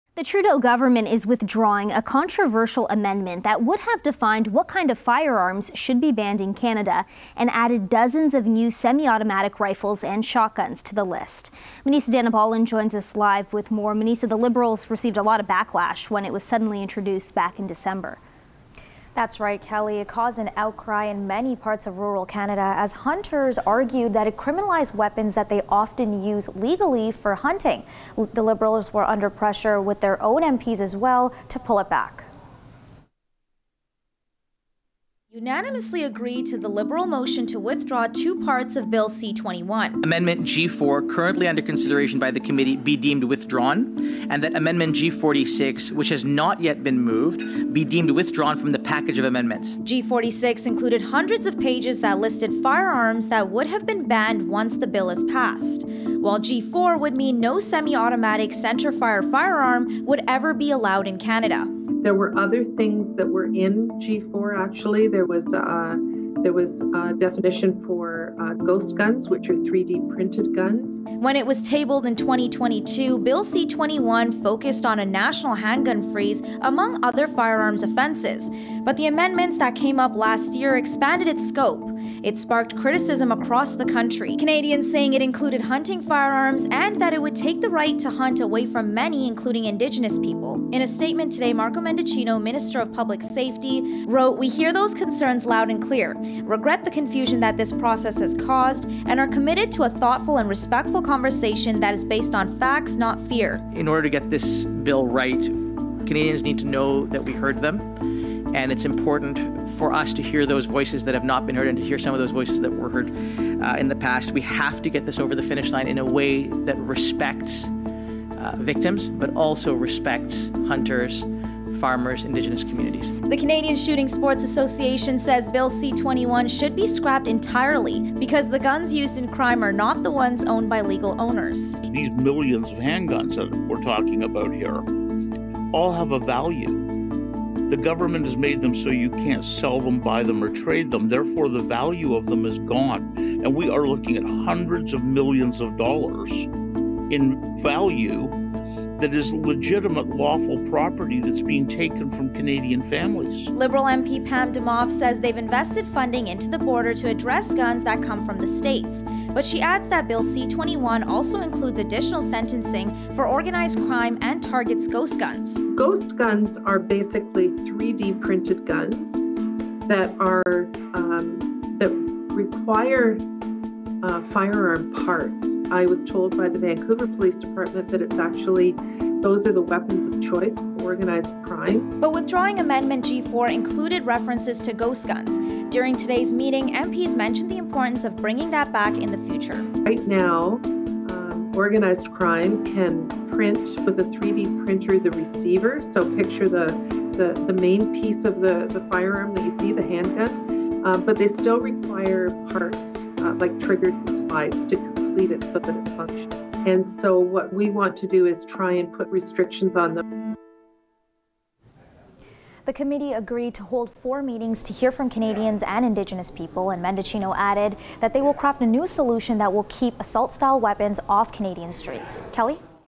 Trudeau government withdraws controversial amendments to Bill C-21
The amendment would have defined what kind of firearms should be banned in Canada and added dozens of new semi-automatic rifles and shotguns to the list. It caused an outcry in many parts of rural Canada, as hunters argued it criminalized weapons they often use legally for hunting.